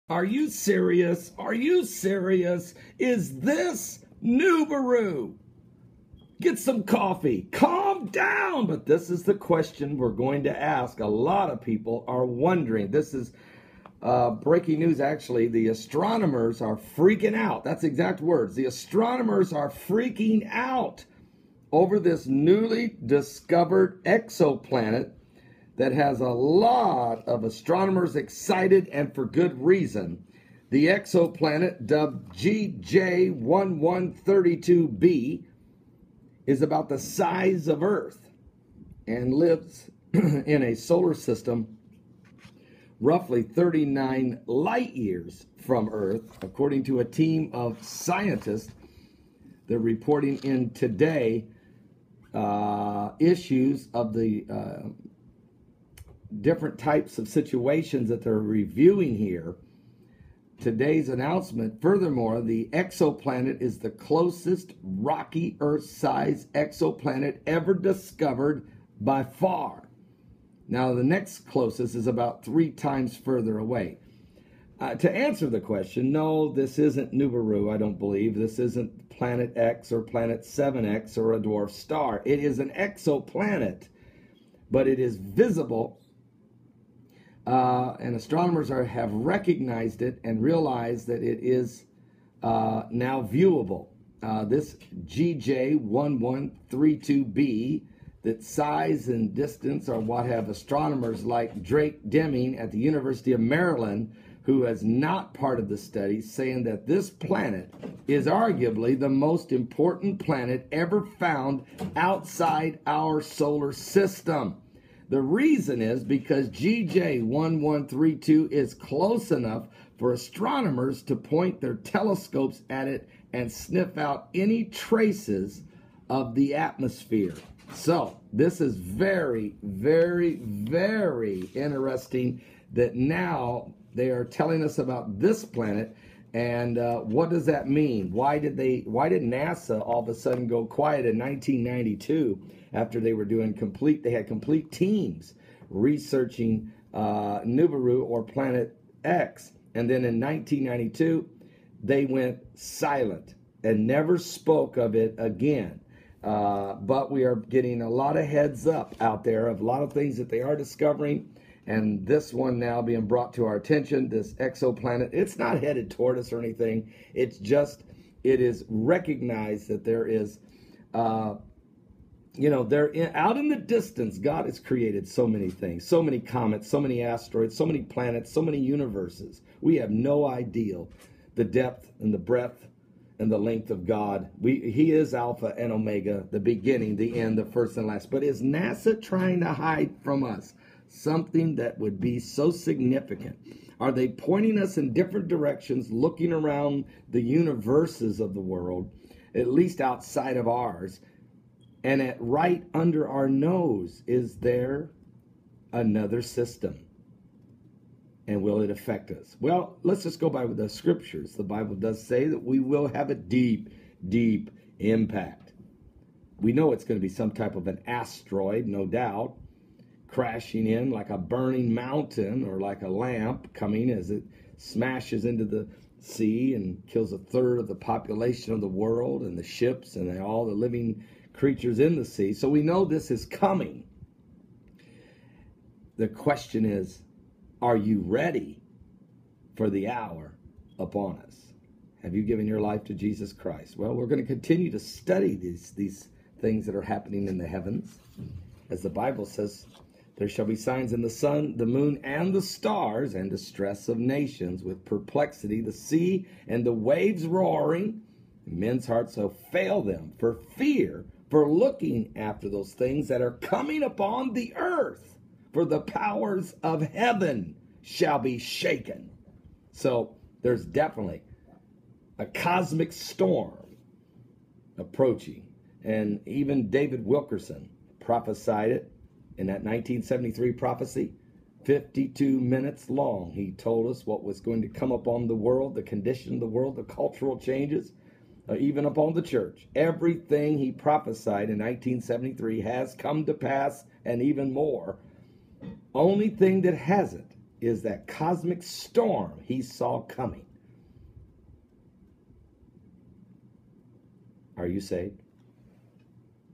BREAKING: "Is This Nibiru"? Earth Size Planet GJ 1132b
Is this Nibiru? Or is NASA preparing us slowly for the dwarf star? Exoplanet GJ 1132b has been discovered also